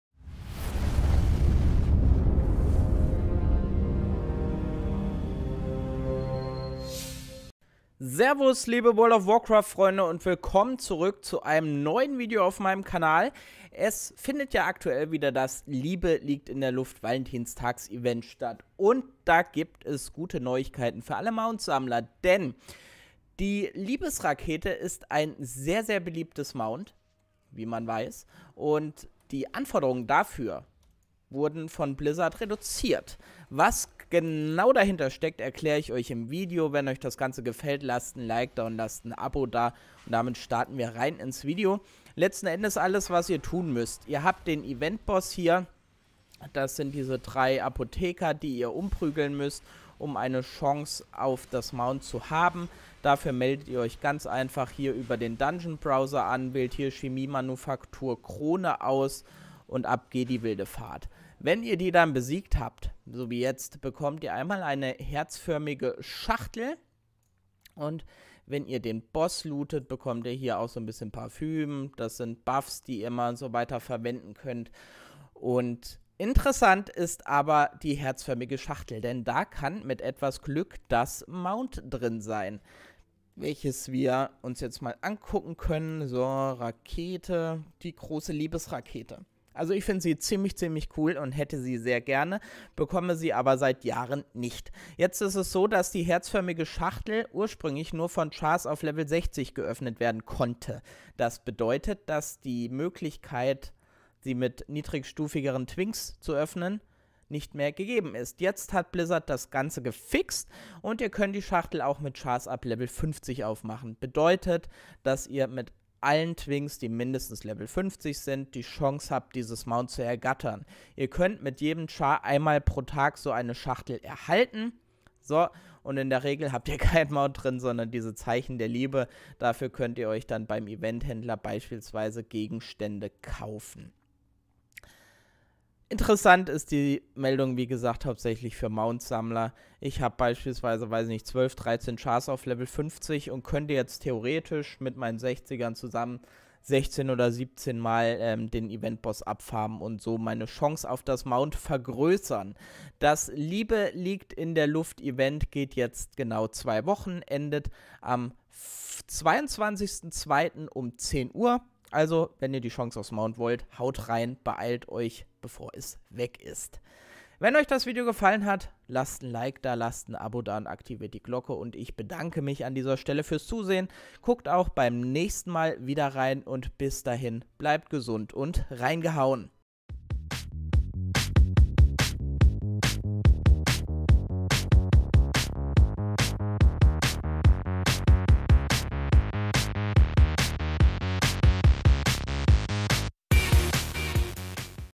Hotfix: Blizzard vereinfacht das Farmen der Liebesrakete mit Twinks [World of Warcraft: Shadowlands]
Ursprünglich hatten nur Charaktere der maximalen Stufe die Chance auf die Große Liebesrakete. Dank einem Hotfix von Blizzard hat sich das nun geändert.